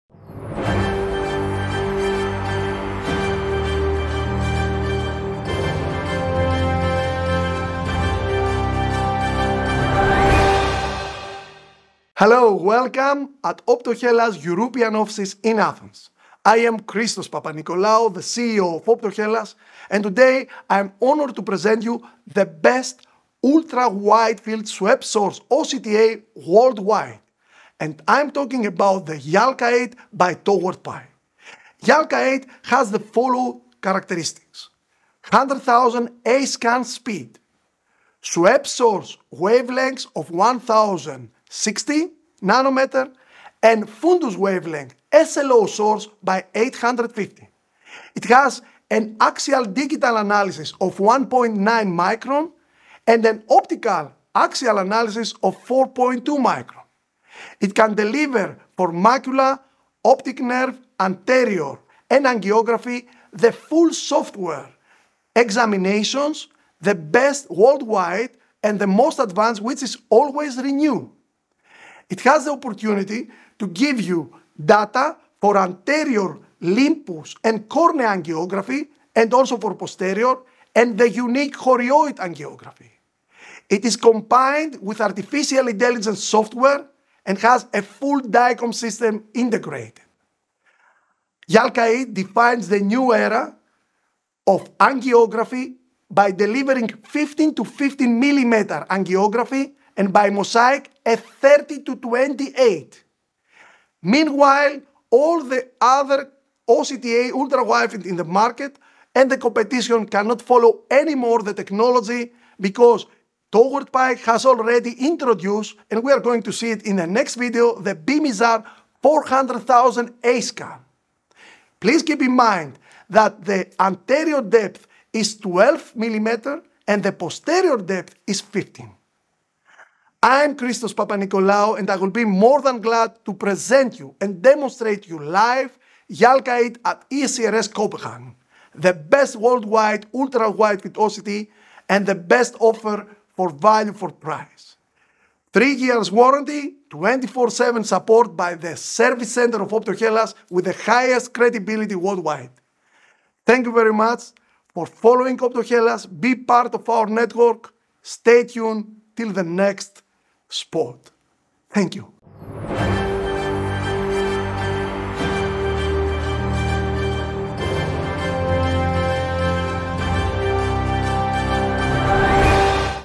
OPTO HELLAS: TOWARD Pi's Yalkaid - The World's Best 100KHz Full-Range SWEPT SOURCE OCTA
Optohellas showcases Yalkaid, the most advanced ultra widefield SS-OCTA worldwide, manufactured by TowardPi.

Yalkaid features unique characteristics for Anterior and Posterior Angiography:

•   Swept source technology 1060nm

•   Fundus Wavelength: SLO 840nm

•   100.000 A-scan/s speed

•   100Hz Ultra-fast eye tracker

•   Axial Digital Analysis: 1.9 μm

•   Optical Axial Analysis: 4.2 μm

•   Wide OCTA Scan up to 15x15mm

•   OCTA Mosaic capability 30x28mm

•   Choroid OCT Angiography

•   Posterior Scan depth 12mm

•   Anterior Scan depth 15mm

•   Unique Anterior OCT Angiography

Yalkaid’s software is always renewed, can be combined with Artificial Intelligence Software and has a Full Dicom System Integrated.

Yalkaid will be presented live at the ESCRS2025 in Copenhagen and is the best offer quality to price value.